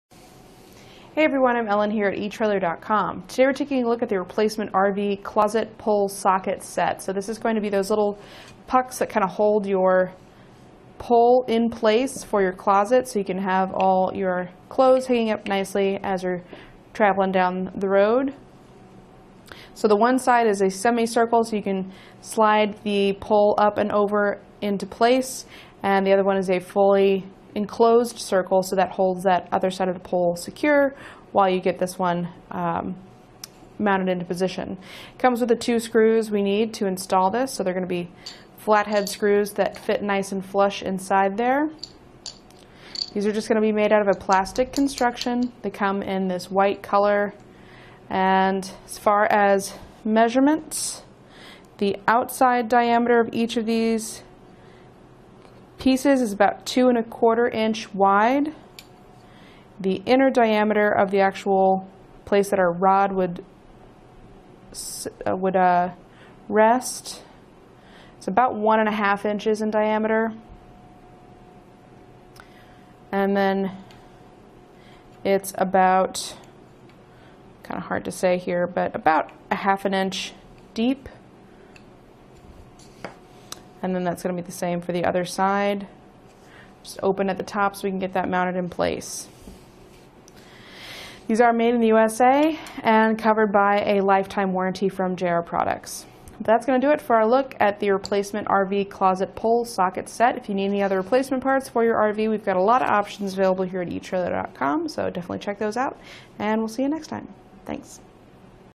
etrailer | Checking Out the Replacement RV Closet Pole Socket Set
Click for more info and reviews of this JR Products RV Door Parts:

Ellen:              Hey everyone. I'm Ellen here at etrailer.com. Today we're taking a look at the replacement RV closet poles socket set. So this is going to be those little pucks that kind of hold your pole in place for your closet so you can have all your clothes hanging up nicely as you're traveling down the road.So the one side is a semicircle so you can slide the pole up and over into place and the other one is a fully enclosed circle so that holds that other side of the pole secure while you get this one mounted into position. It comes with the two screws we need to install this. So they're going to be flat head screws that fit nice and flush inside there.

These are just going to be made out of a plastic construction. The come in this white color. And as far as measurements, the outside diameter of each of these pieces is about two and a quarter inch wide. The inner diameter of the actual place that our rod would rest, it's about one and a half inches in diameter. And then it's about kind of hard to say here, but about a half an inch deep and then that's going to be the same for the other side, open at the top so we can get that mounted in place.These are made in the USA and covered by a lifetime warranty from Jr.

Products. That's going to do it for our look at the replacement RV closet poles socket set. Thanks.